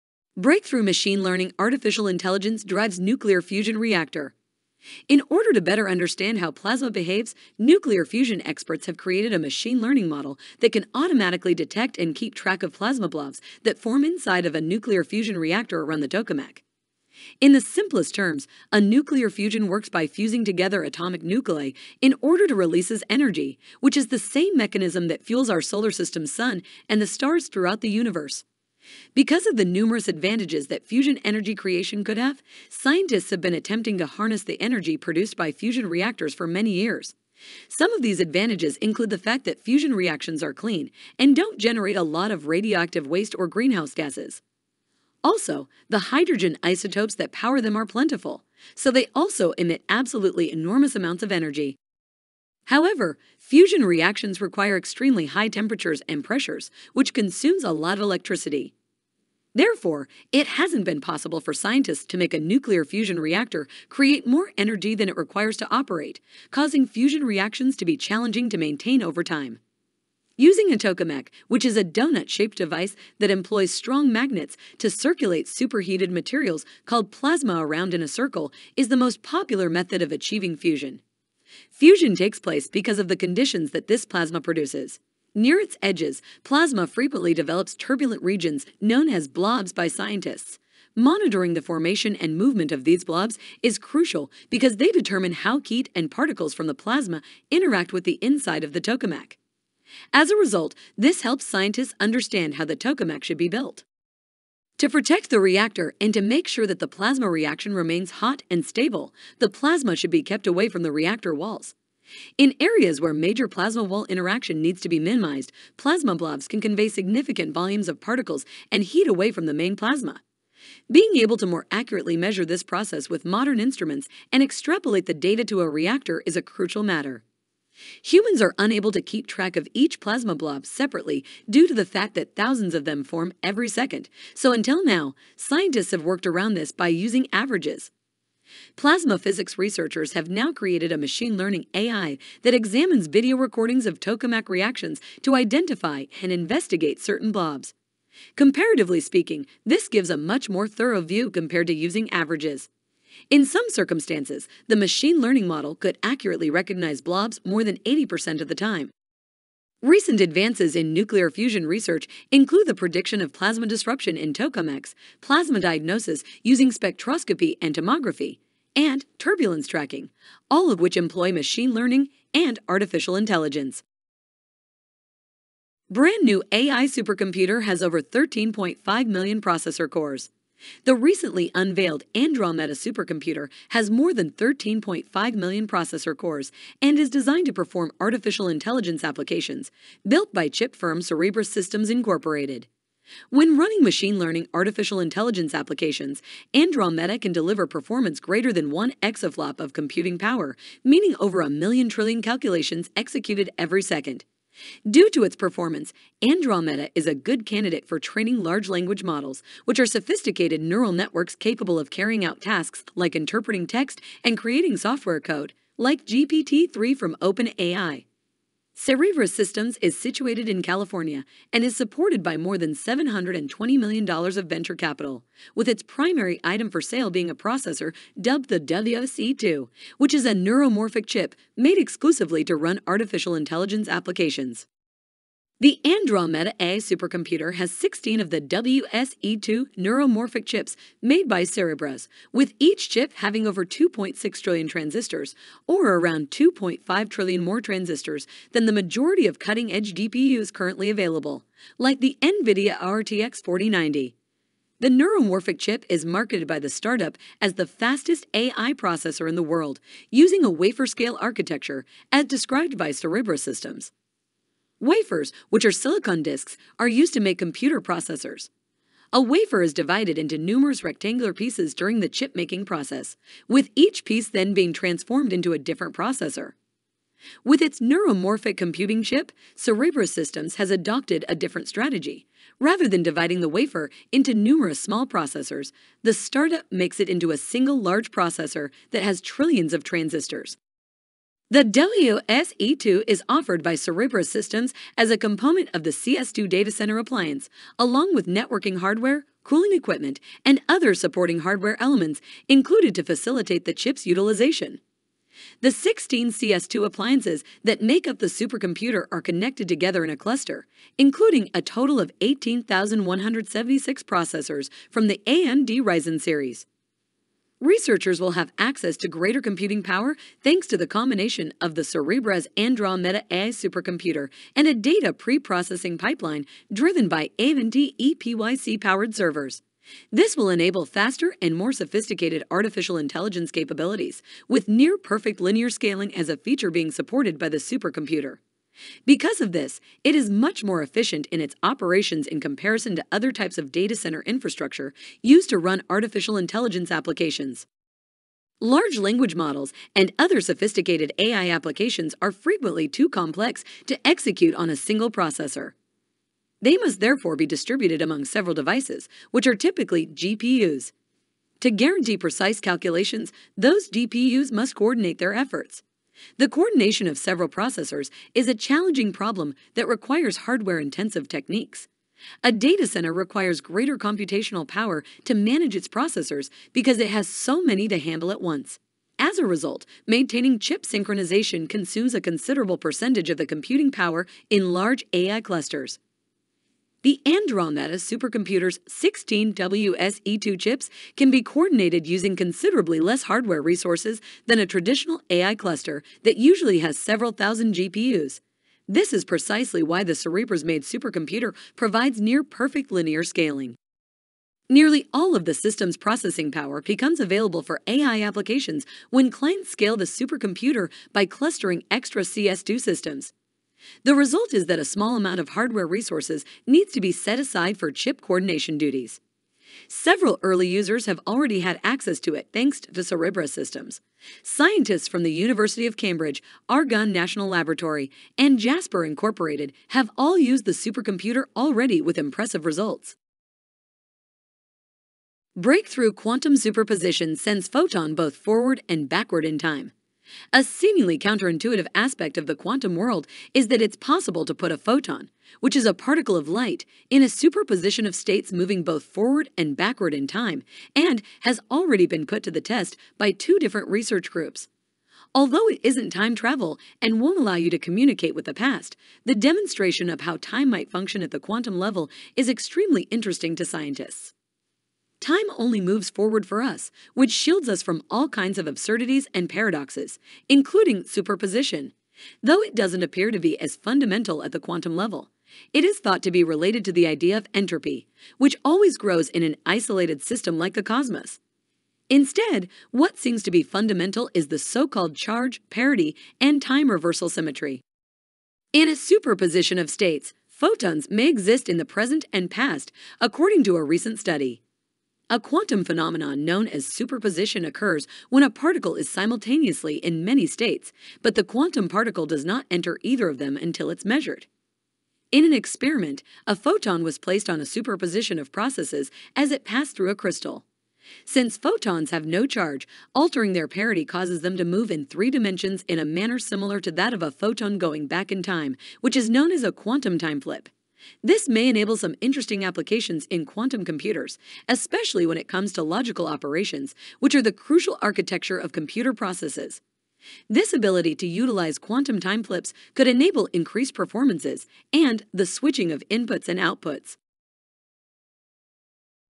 How This Breakthrough Machine Learning AI Drives a Nuclear Fusion Reactor
Nuclear fusion scientists used a machine learning artificial intelligence model to autonomously monitor and control the plasma blobs around the nuclear fusion reactor's tokamak, bringing fusion energy closer than ever before. New AI supercomputer with over 13.5 million processor cores comes in 16 pieces to form a cluster using WRE-2 neuromorphic chip to achieve 1 exaflop of computation. Quantum physics researchers have carried out a successful experiment to superposition a photon, allowing the photon to simultaneously travel both forwards and backwards in time.

Tech News Timestamps:
0:00 Breakthrough Machine Learning AI Nuclear Fusion Reactor
2:55 New AI Supercomputer With 13.5+ Million Cores
7:15 Breakthrough Quantum Physics Time Travel